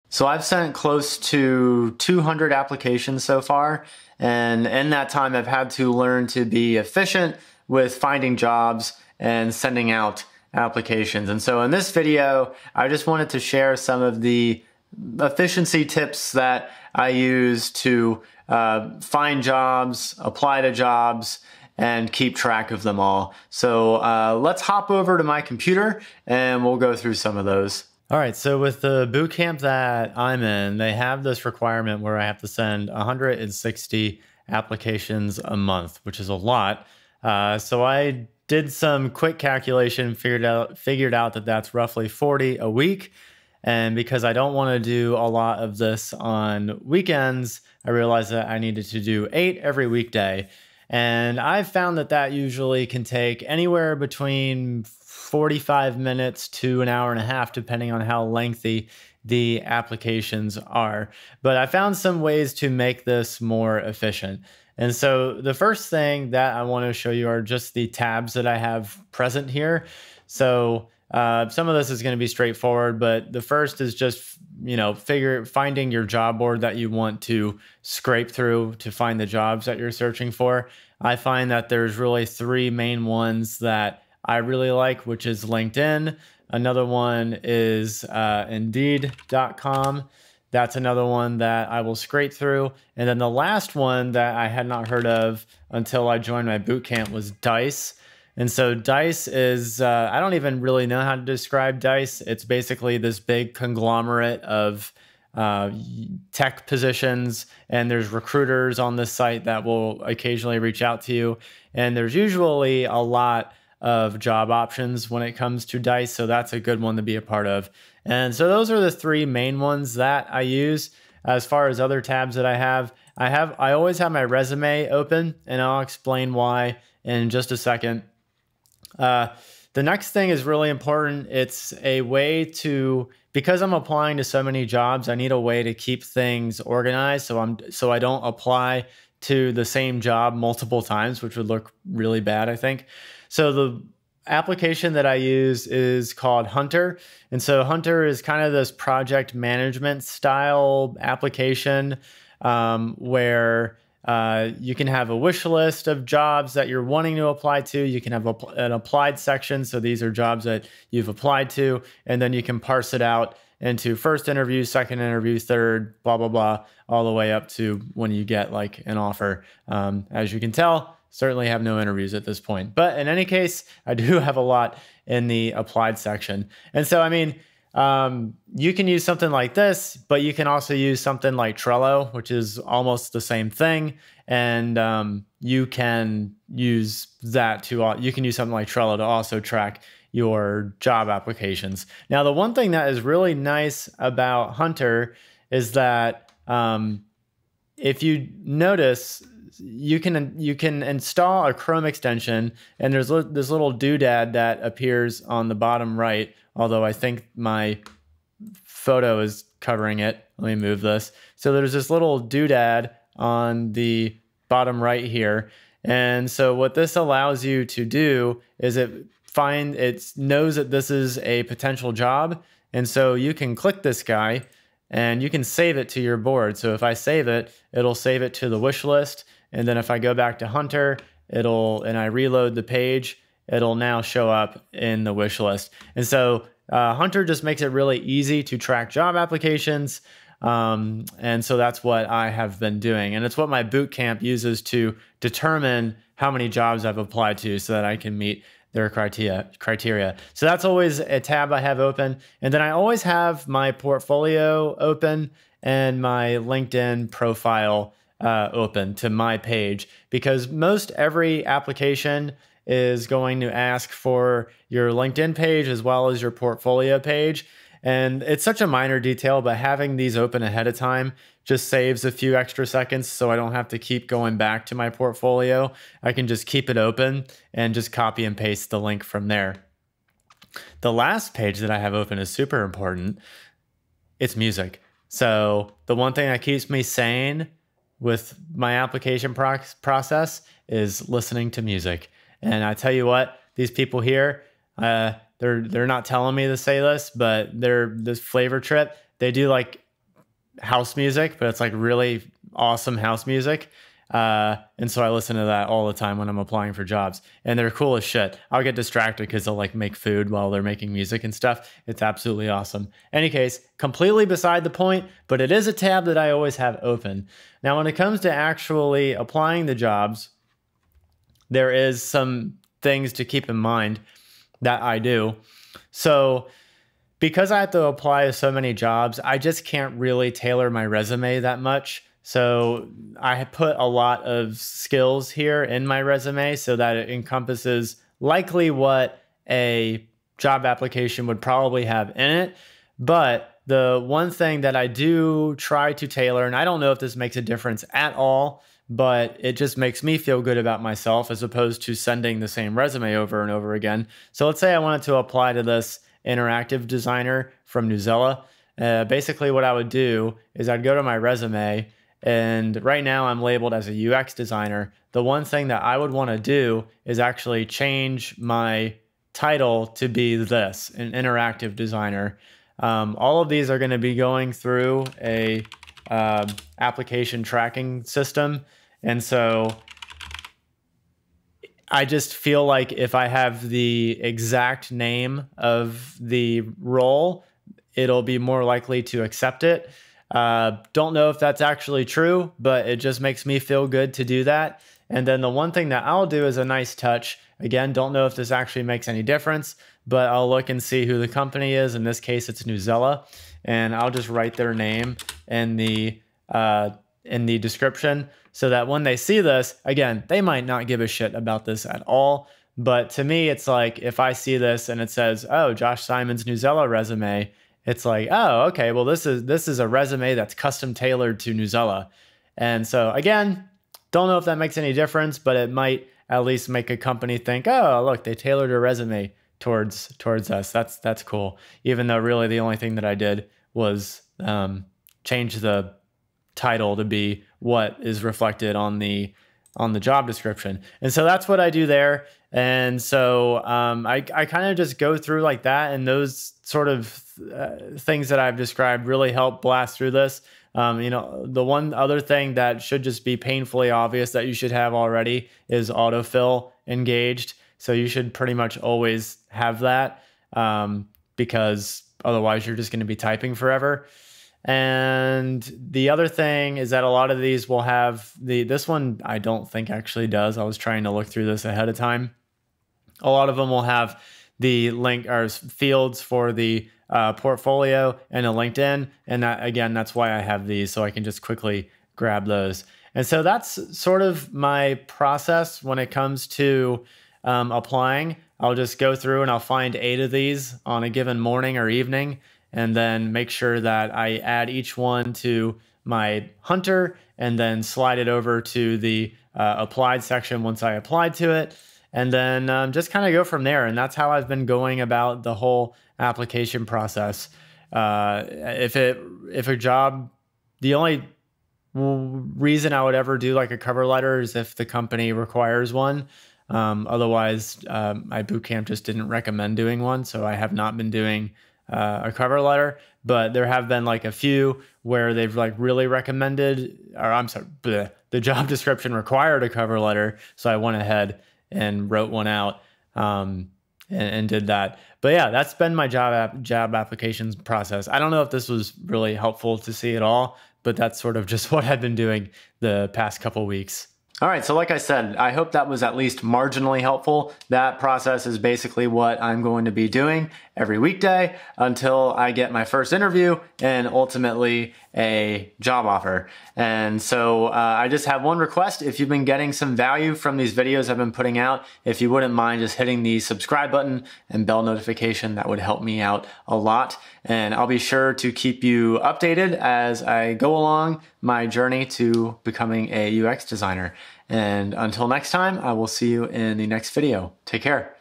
How I Efficiently Apply to UX Design Jobs
My bootcamp requires me to send 160 applications per month, so I've developed little tips that help me with the application process.